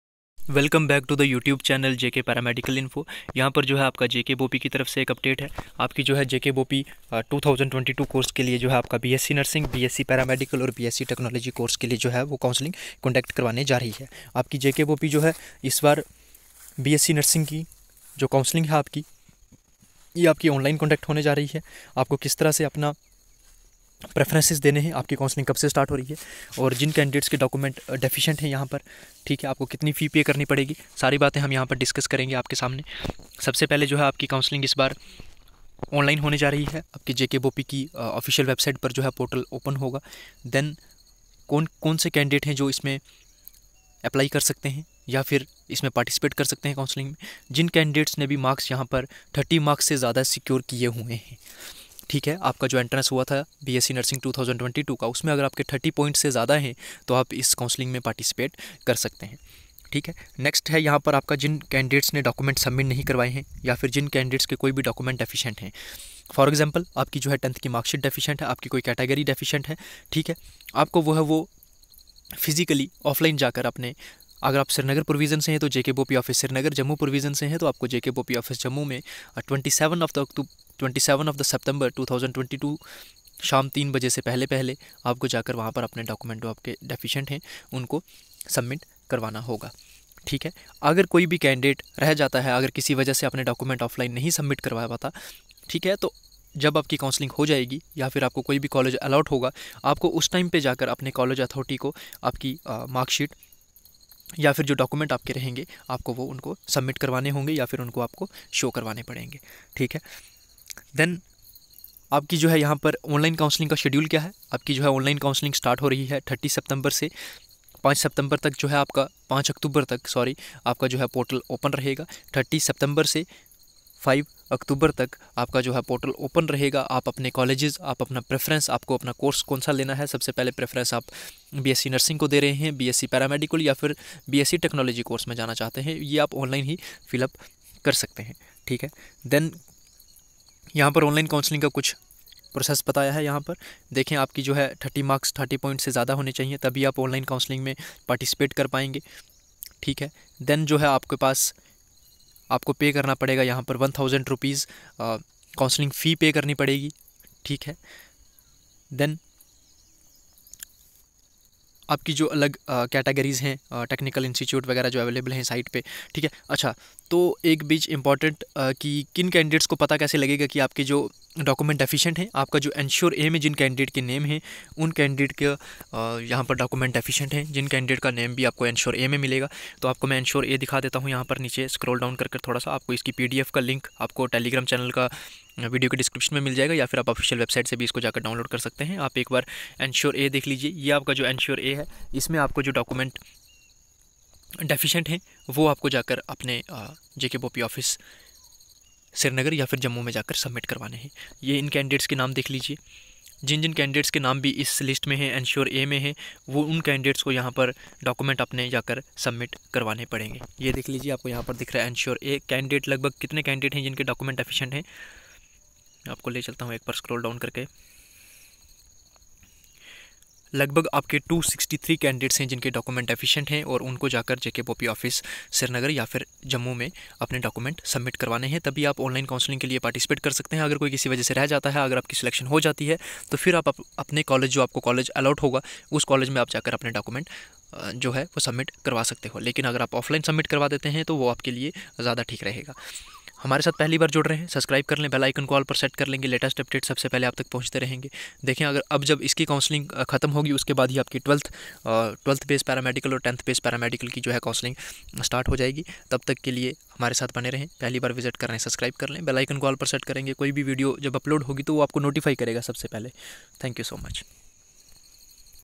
Jkbopee online counseling start Bsc nursing 2022 || Bsc Para medical || Bsc Technology
Conduct of online counseling of eligible candidates for admission to B.Sc. Nursing, B. Sc. Paramedical and B.Sc. Technology Courses-2022

jkbopee bscnursing counselling schedule
jkbopee bscnursing counselling
jkbopee bscnursing online counselling
bscnursing bscparamedical and bsc Technology Courses counselling schedule
bscnursing, bscparamedical and bsc Technology Courses.